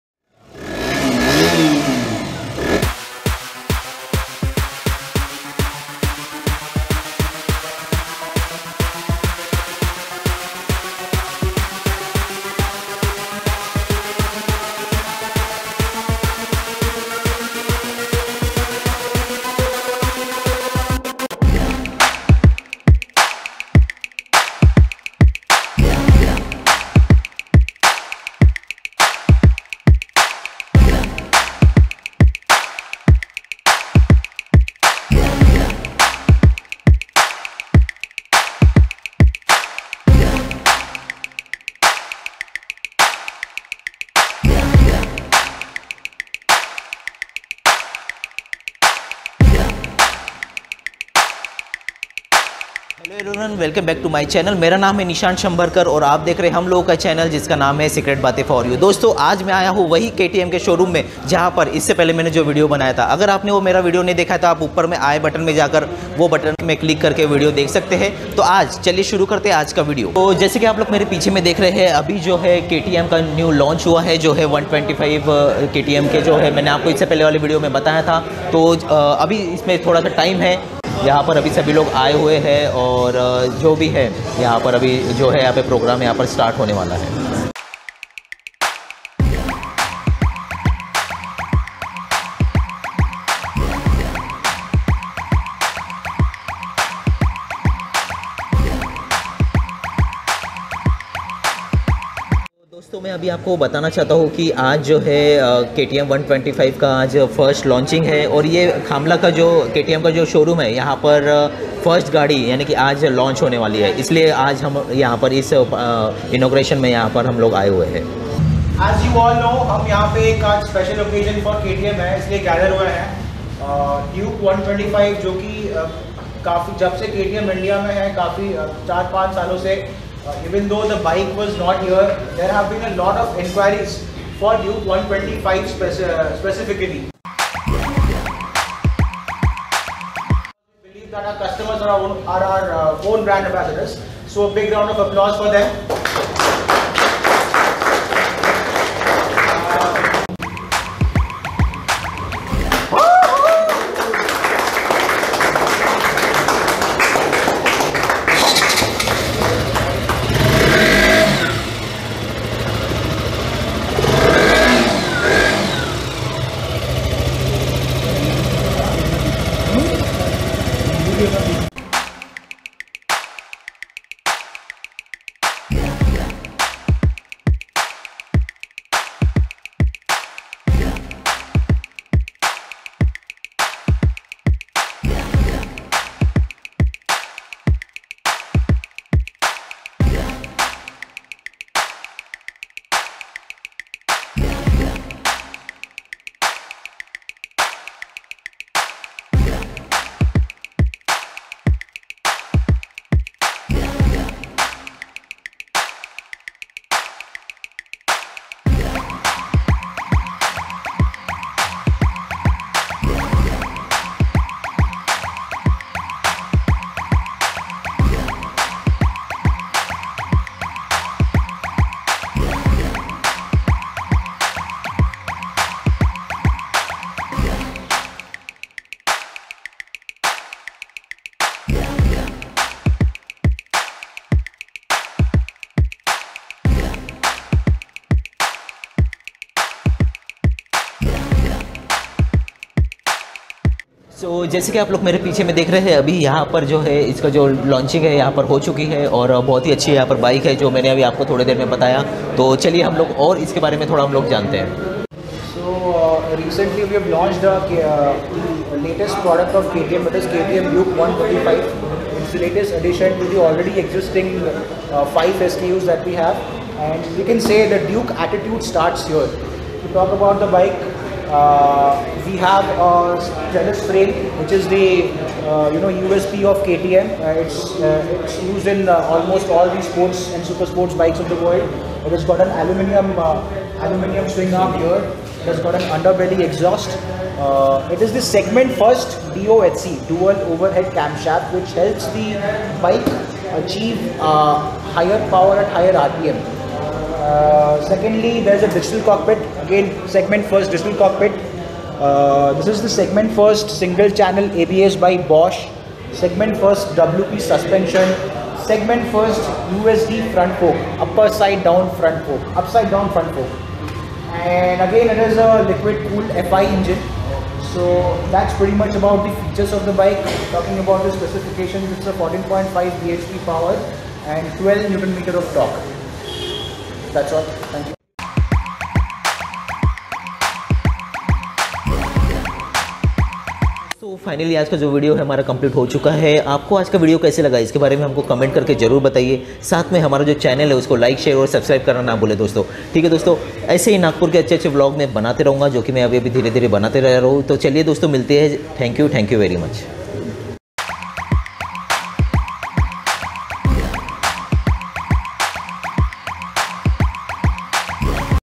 KTM DUKE 125 FULL REVIEW NAGPUR || KTM LAUNCHING PARTY NAGPUR SHOWROOM || VLOG#33
ktm duke 125 launched in nagpur showroom. khamla branch.  NAGPUR  KTM.is video me maine ktm 125 k bare me batya hai jo just launch hue hai hope ki apko video jarur pasand ayega. video ko like subscribe karna na bhule thanks